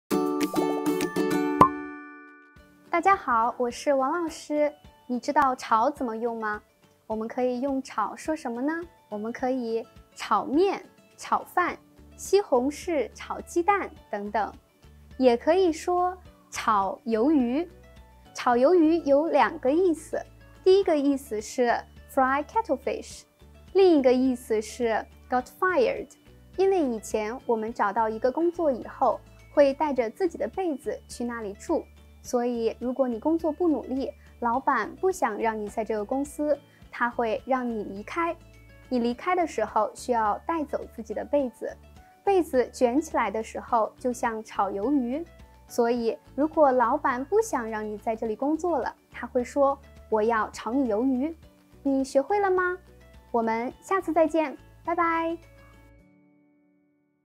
Learn Chinese Vocabulary: Getting fired
Have you ever heard of another humorous way of expressing "getting fired"? Today we look at what someone might say to you in Chinese if you get fired in China.

Dive into an unforgettable language learning experience across our vibrant campuses in Beijing, Shanghai, Shenzhen, Chengdu, and Suzhou.